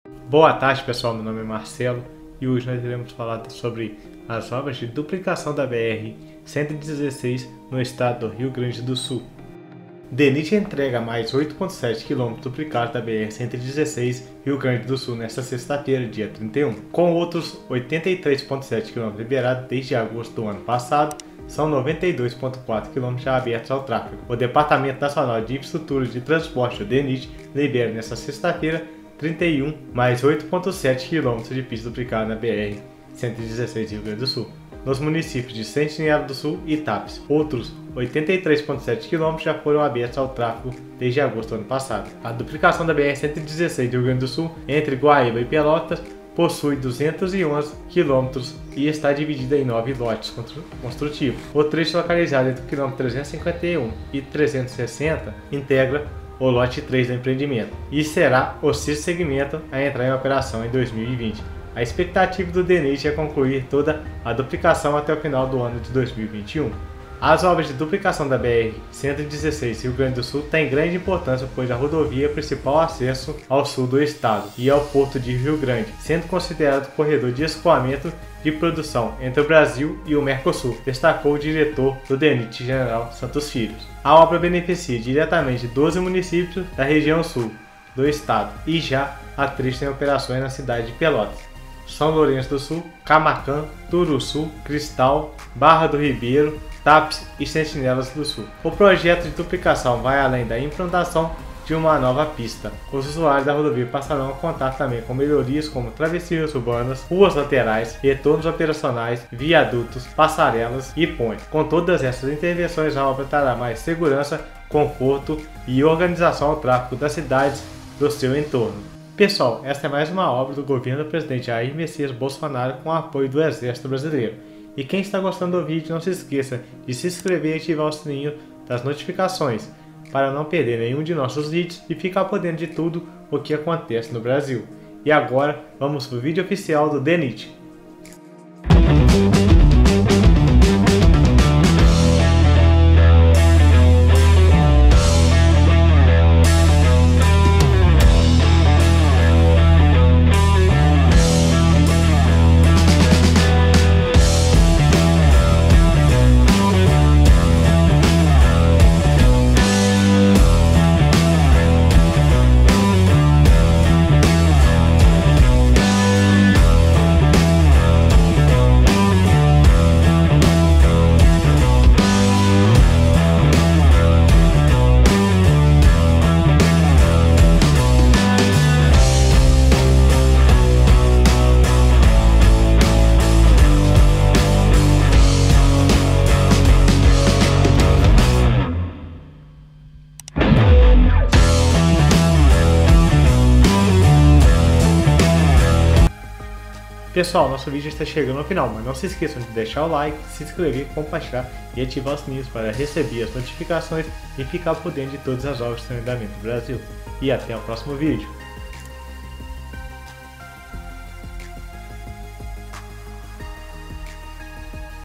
URGENTE !!! EXERCITO BRASILEIRO ACABA DE ENTREGAR MAIS 8,7 KM DUPLICAÇÃO DA BR 116 RS GOV.BOLSONARO
“As obras de duplicação da BR-116/RS têm grande importância pois a rodovia é a principal via de acesso ao Sul do Estado e ao Porto de Rio Grande, sendo considerada corredor de escoamento de produção entre o Brasil e o Mercosul”, destacou o diretor-geral do DNIT, general Santos Filho.

A obra beneficia diretamente 12 municípios da região Sul do Estado e já há trechos em operação nas cidades de Pelotas, São Lourenço do Sul, Camaquã, Turuçu, Cristal, Barra do Ribeiro, Tapes e Sentinela do Sul.

O projeto de duplicação vai além da implantação de uma nova pista. Os usuários da rodovia passarão a contar também com melhorias como travessias urbanas, ruas laterais, retornos operacionais, viadutos, passarelas e pontes.  Com todas essas intervenções, a obra trará mais segurança, conforto e organização ao tráfego das cidades do seu entorno.

Confira os locais liberados e o andamento dos serviços nos nove lotes de obra:

Em operação:

Pelotas: 19,65 quilômetros (liberados em agosto de 2019)

São Lourenço do Sul: 12,25 quilômetros (liberados em agosto de 2019)

Camaquã: 15,1 quilômetros (liberados em agosto de 2019)

Turuçu: 7,7 quilômetros (liberados em março de 2020)

São Lourenço do Sul: 5,1 quilômetros (liberados em março de 2020)

Cristal: 9,2 quilômetros (liberados em março de 2020)

Barra do Ribeiro: 5 quilômetros (liberados em abril de 2020)

Tapes/Sentinela do Sul: 9,7 quilômetros (liberados em junho 2020)

Evolução dos serviços nos nove lotes construtivos:

Lotes 01 e 02: km 300 ao km 325 (Guaíba a Barra do Ribeiro) e km 325 ao km 351 (Barra do Ribeiro a Sentinela do Sul/Tapes) – 68,5%executado

Lote 03: km 351 ao km 373 (Sentinela do Sul/Tapes) – 76,88% executado

Lote 04: km 373 ao km 397 (Sentinela do Sul/Tapes – Camaquã) – 91,18% executado

Lote 05: km 397 ao km 422 (Camaquã – Cristal) - 48,84% executado

Lote 06: km 422 ao km 448 (Cristal – São Lourenço do Sul) - 66,81% executado

Lote 07: km 448 ao km 470 (São Lourenço do Sul) - 72,95% executado

Lote 08: km 470 ao km 489 (São Lourenço do Sul –Turuçu) – 84,75% executado

Lote 09: km 489 ao km 511 (Turuçu - Pelotas) – 89,21% executado

Coordenação-Geral de Comunicação Social – DNIT